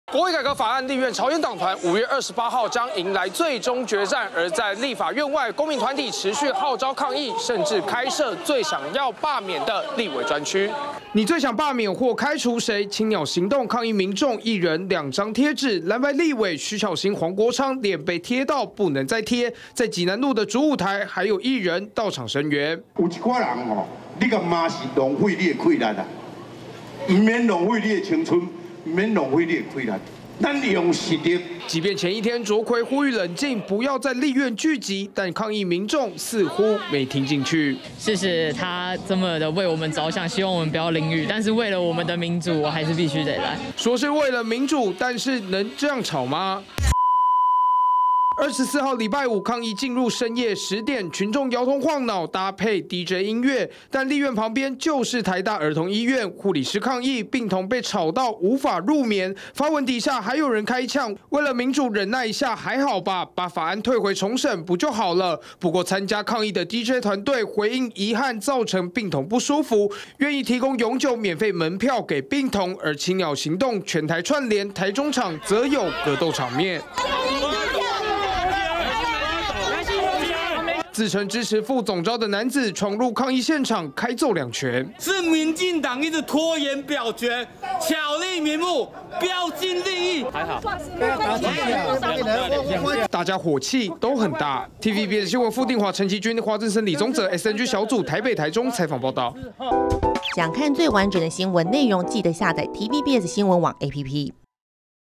「青鳥行動」濫權專區 黃國昌.徐巧芯登榜｜TVBS新聞
國會改革法案，立院朝野今(28)天最終決戰，在立法院外，"青鳥行動"號召群眾包圍抗議持續到深夜。而在青島東路的第二抗議現場，這次還特別設置"濫權立委專區"，邀請民眾選出最想罷免的藍白立委，分別是徐巧芯和黃國昌。